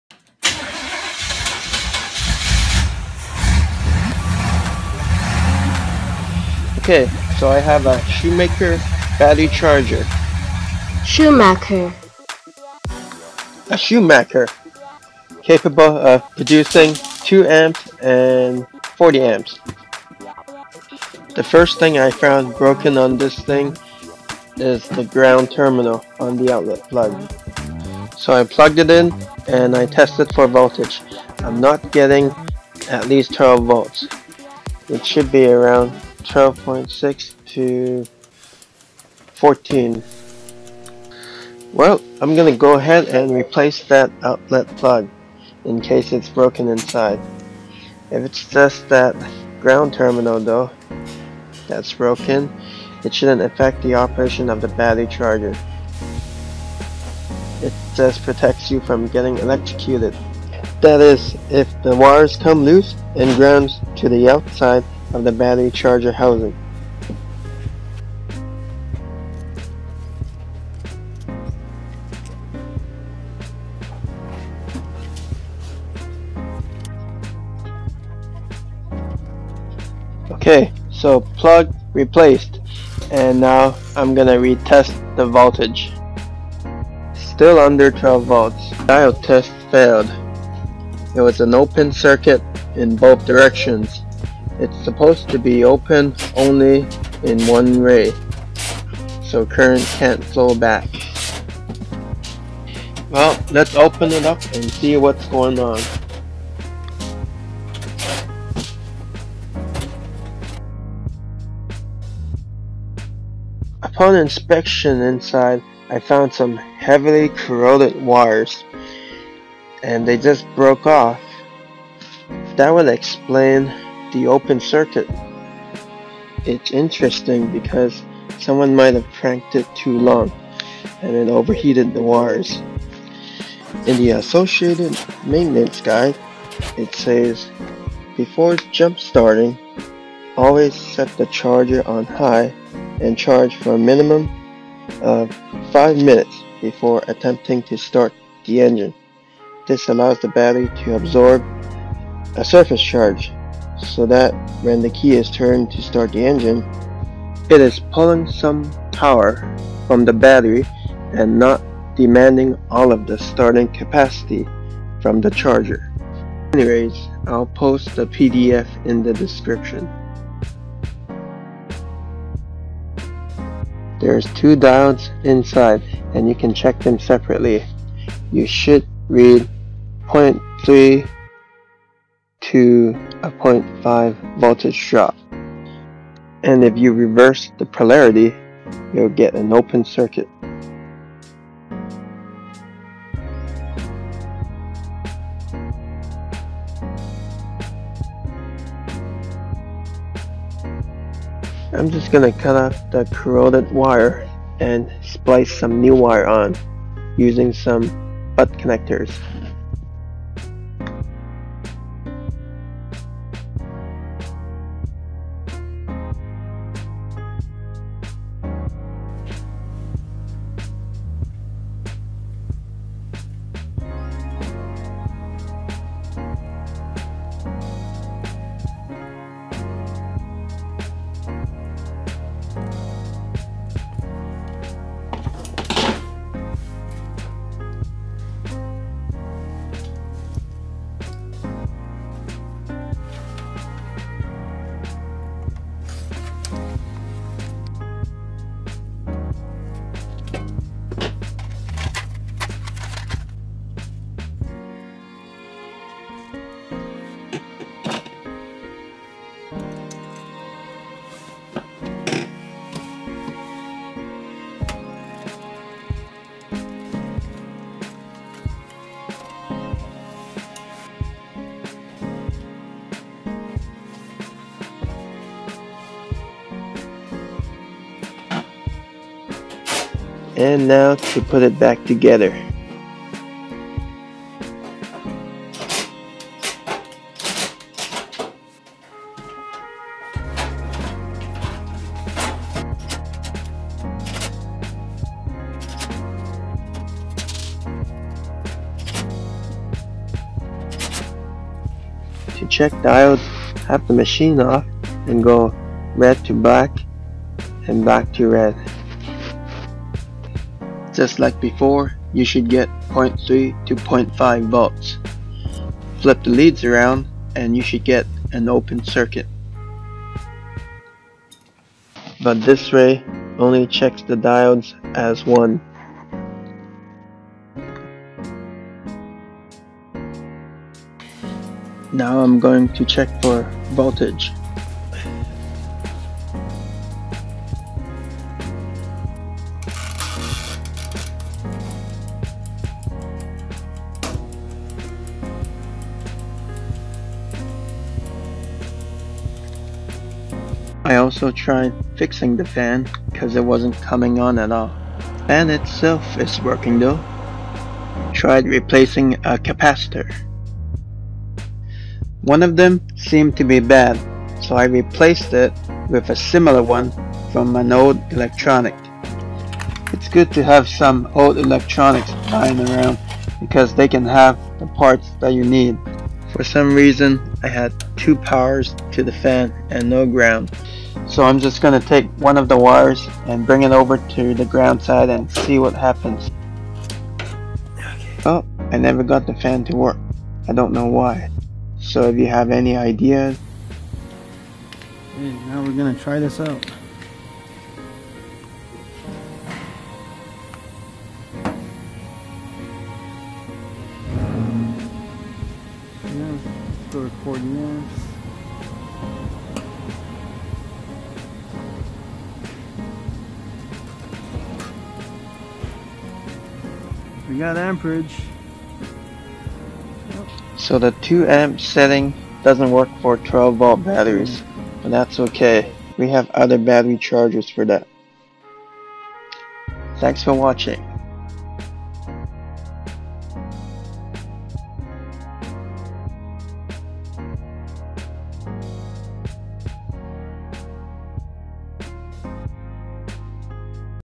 Schumacher Battery Charger Repair, Part I
The charger stopped working, no amperage at all. So the shop decided to buy a new charger, at least 300$ for these chargers. Well, why not fix it? So I did.
I actually measured 13.6V after it was fixed. Every setting except the 2A.
Also that battery at the end of the video probably wasnt the best to show charging amperage because it was bad, sorry. but at least I showed that the charger was working. A bad battery wont take much of a charge.